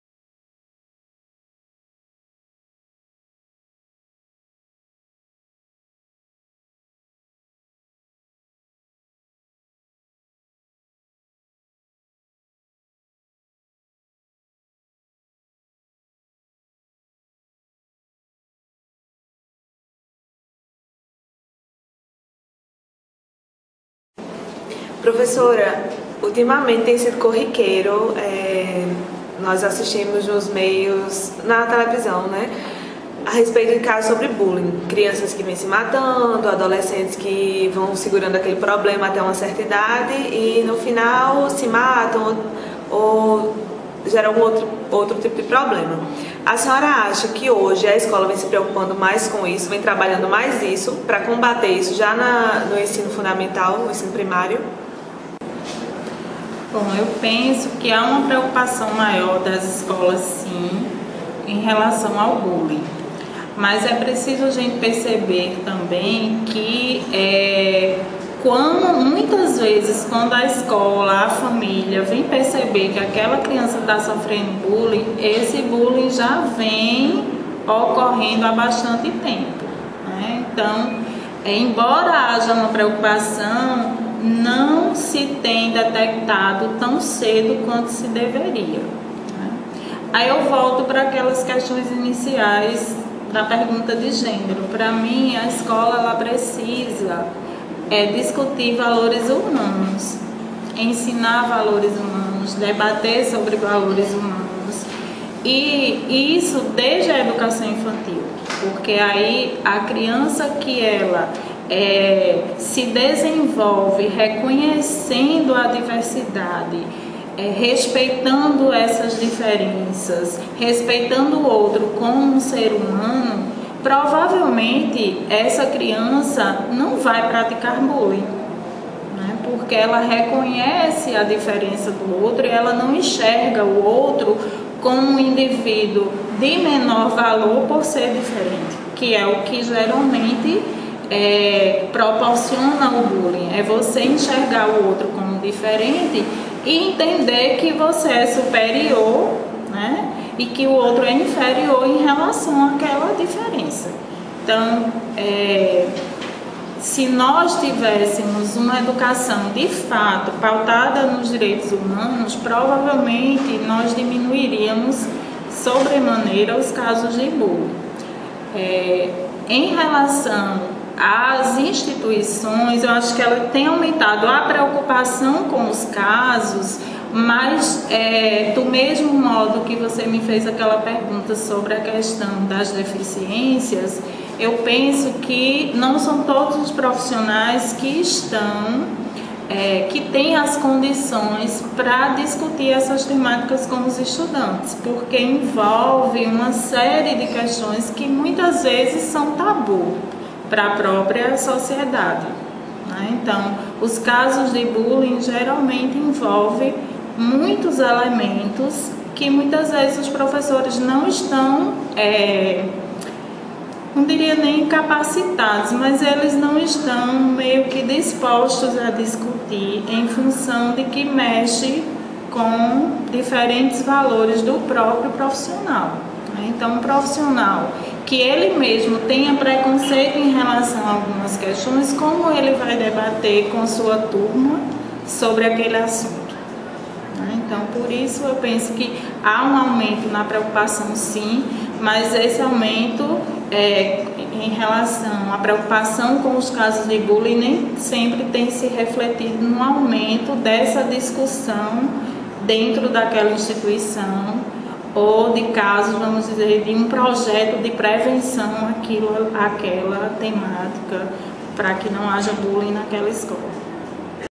V Colóquio Infância e Adolescência, com Ana Luisa Nogueira de Amorim - 13
13 - Bullying nas escolas
- Neste vídeo, a professora comenta a preocupação das escolas quanto ao bullying e ressalta a importância de discutir valores humanos desde a educação infantil.
- 13º vídeo do V Colóquio sobre Infância e Adolescência, realizado no dia 13 de outubro de 2016 com a professora Ana Luisa Nogueira de Amorim (coordenadora do curso de Pedagogia na modalidade a distância), organizado pelas alunas Carolina Jurado, Luíza Araújo, Maria Clara Lima e Mary Jéssica para a disciplina Jornalismo, Cidadania e Direitos Humanos, do curso de Jornalismo da UFPB. Supervisão: Carmélio Reynaldo Ferreira.